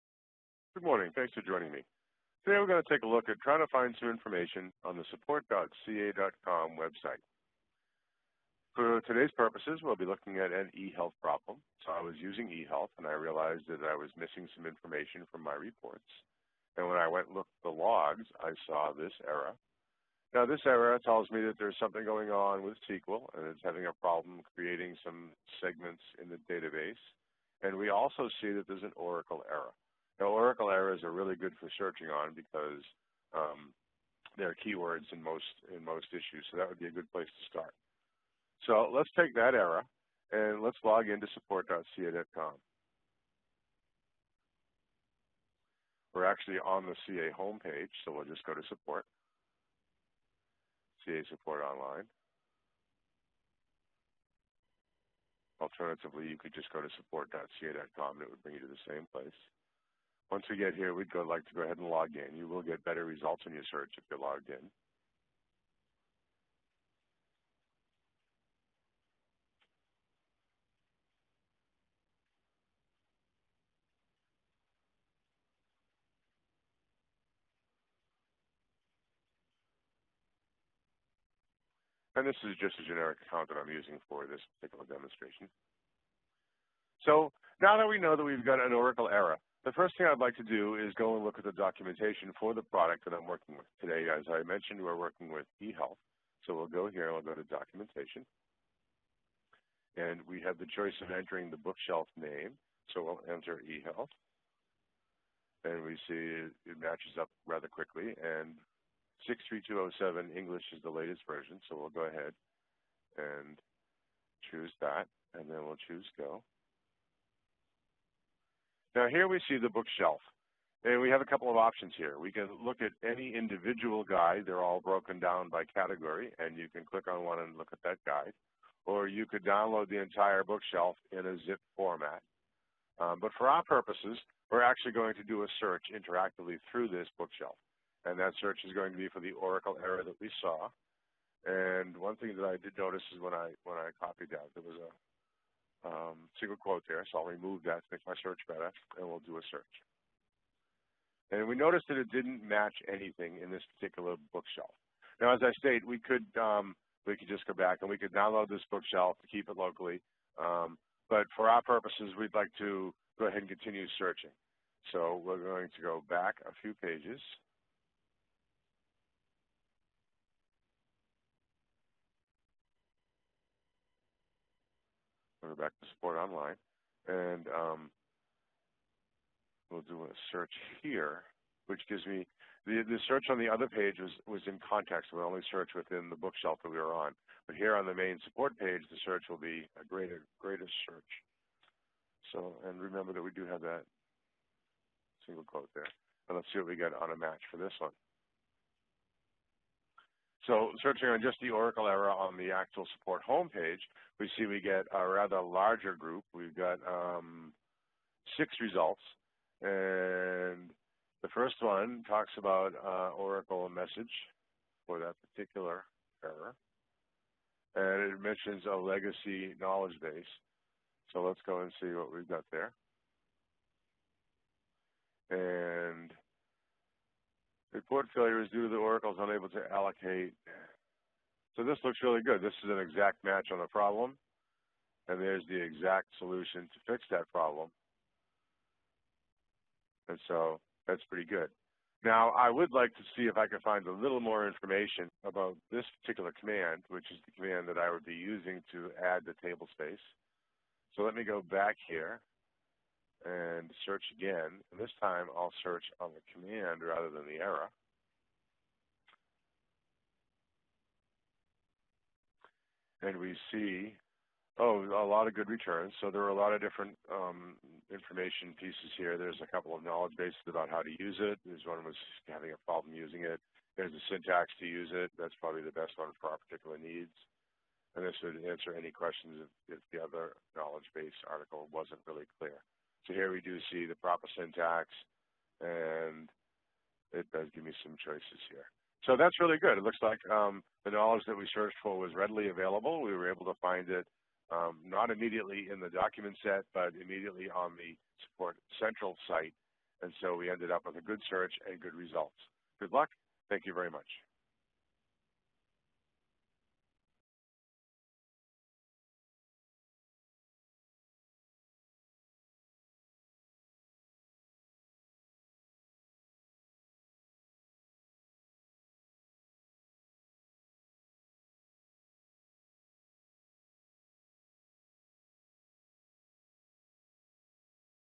How to Find Knowledge Articles from Error Messages
Support engineer Rich Vernon walks through how to search the technical documentation within both the bookshelf and knowledge document articles on ca.com to find information related to error messages.